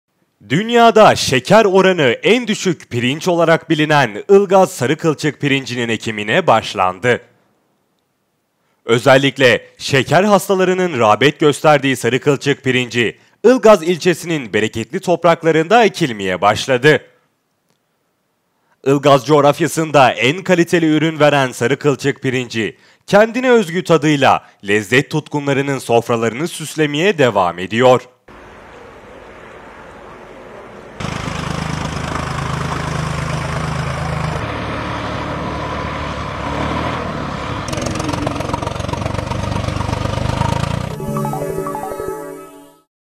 Şeker oranı en düşük Ilgaz Sarıkılçık Pirinci’nin ekimine başlandı!
Dünya’da şeker oranı en düşük Pirinç olarak bilinen Ilgaz Sarıkılçık pirincinin ekimine başlandı.